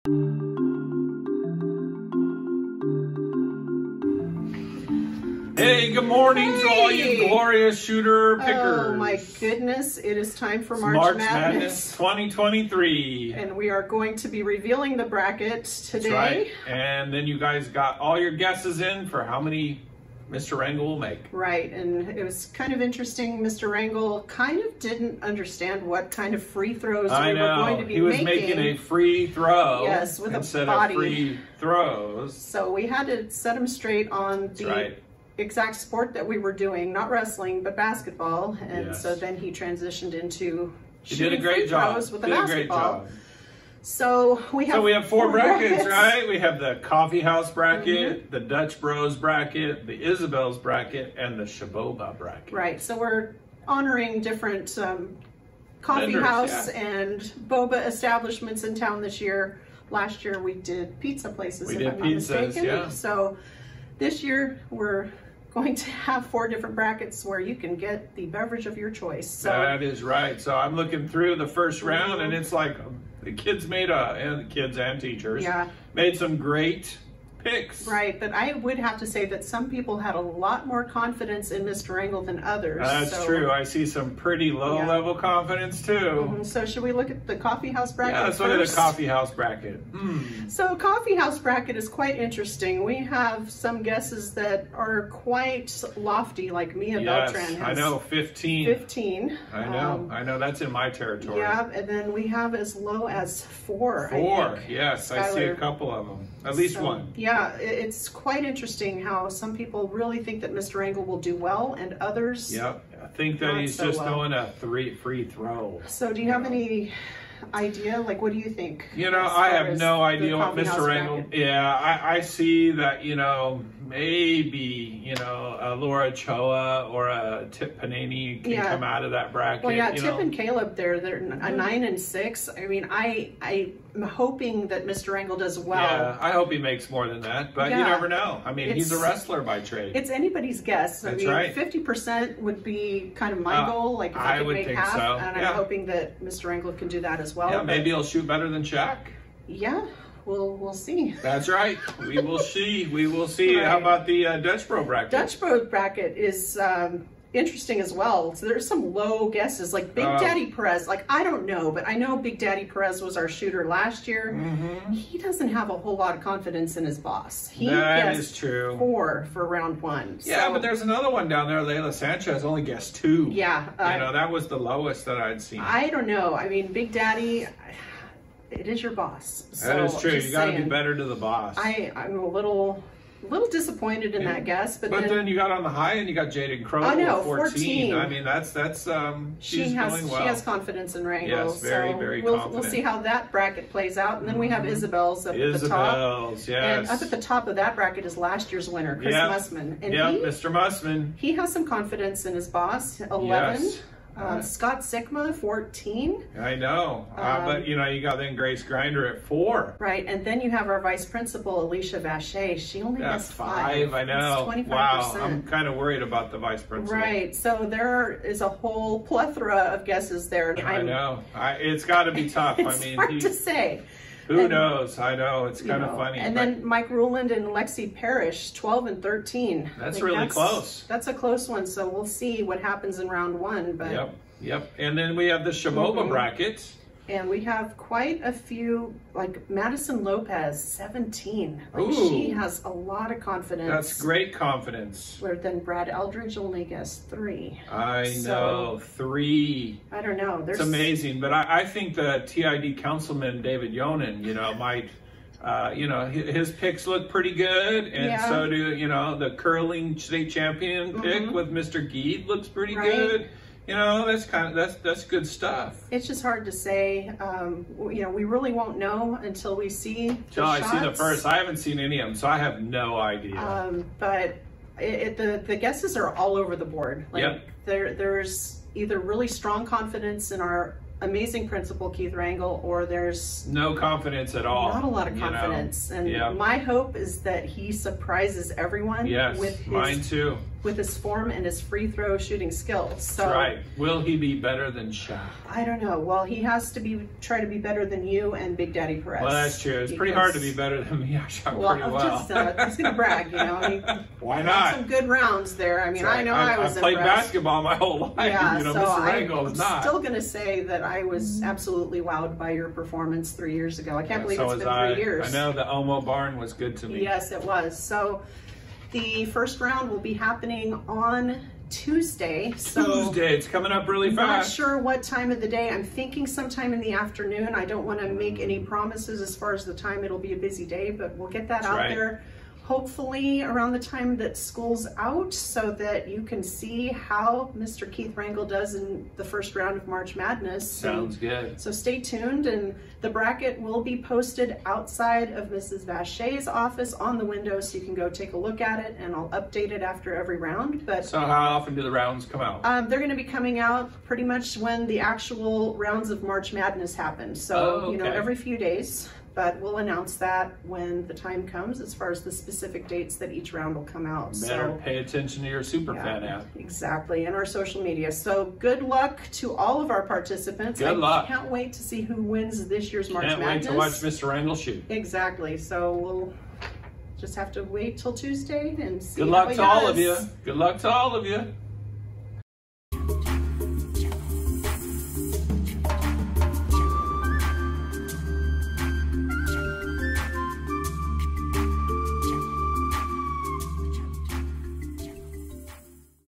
March Madness 2023 Bracket Reveal
We are revealing the bracket for March Madness! See the picks AND listen to our analysis of Round 1's prospects.